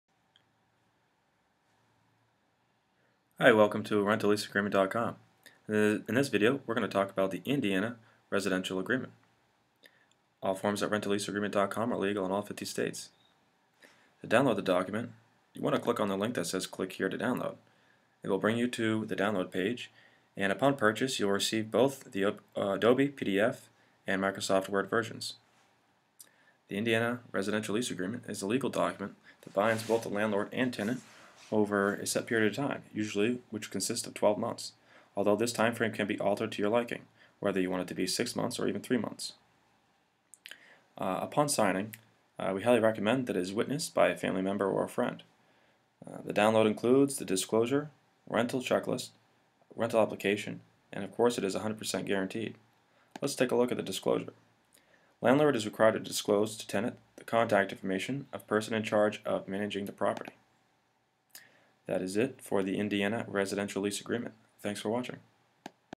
Indiana Residential Lease Agreement
The Indiana Residential Lease Agreement is a legal document that binds a Landlord and Tenant over a set period of time for the renting of one's property. The agreement can be made for any time frame legal under law in the State of Indiana, but the most common term is 12 months.
The Landlord or Representative and Tenant must authorize the contract for it to be legally binding and it is highly advised that the form be witnessed or notarized.